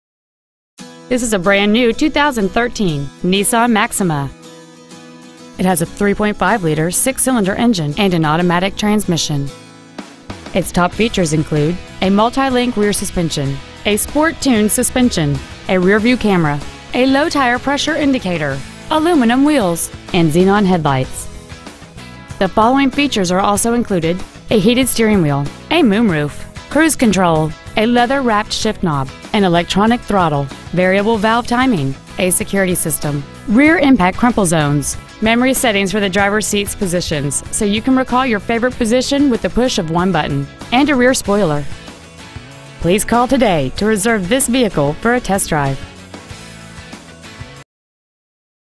2013 Maxima Fort Worth TX
We are proud to present this 2013       Maxima .

 Year : 2013
 Make :
 Model : Maxima
 Engine : 3.5L DOHC 24-valve SMPI V6 engine
 Trans . : Automatic
 Exterior : White
 Miles : 6
 Interior : Black

 2013       Maxima Dallas TX